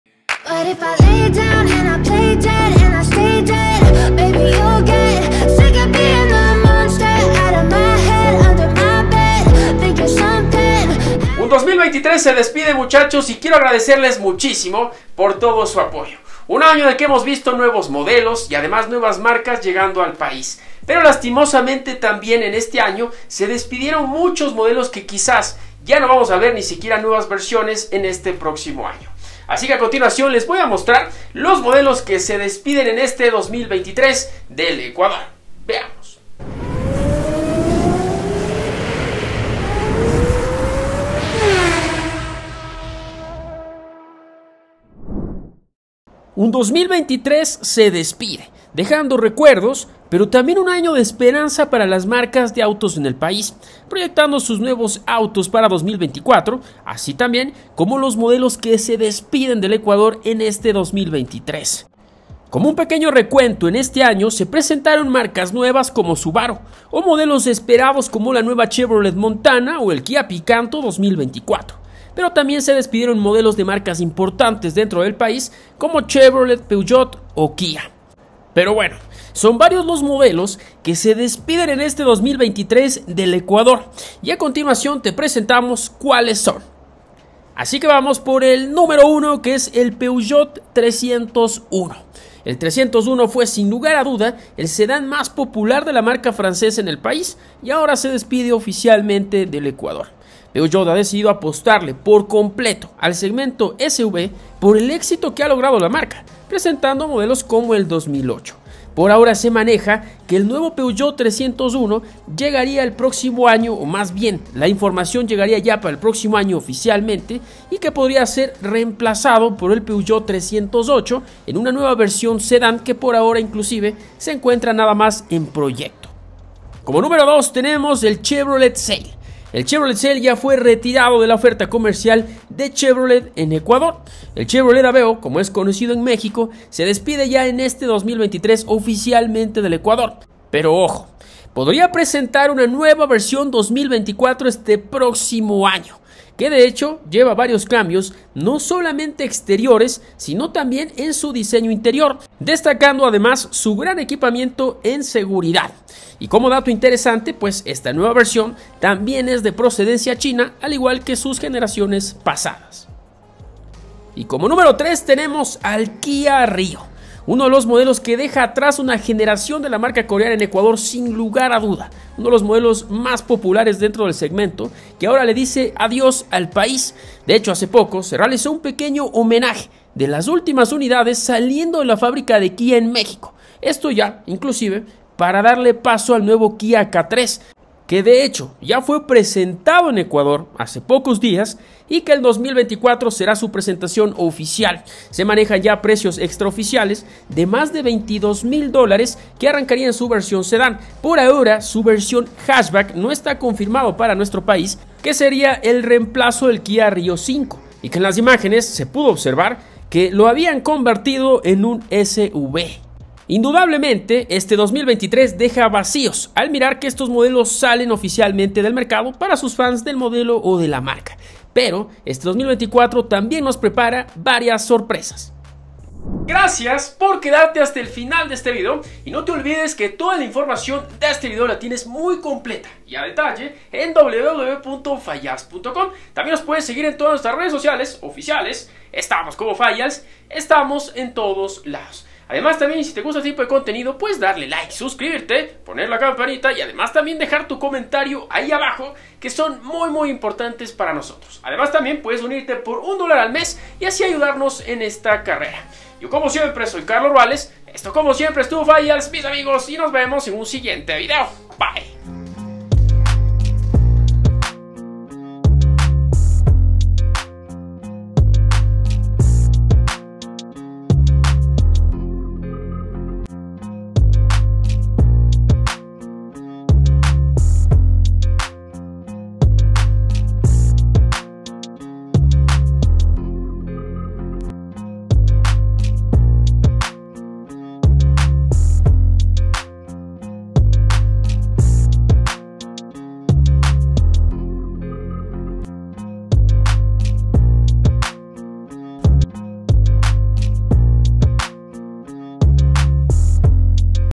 Los 3 CARROS que SE DESPIDEN del ECUADOR en 2023 😕 | Fayals
Estos son los 3 AUTOS que SE DESPIDEN del ECUADOR en 2023. Gracias por todo el apoyo en este 2023, un abrazo para ustedes y muchas bendiciones para sus familias.

Deja tu comentario o sugerencia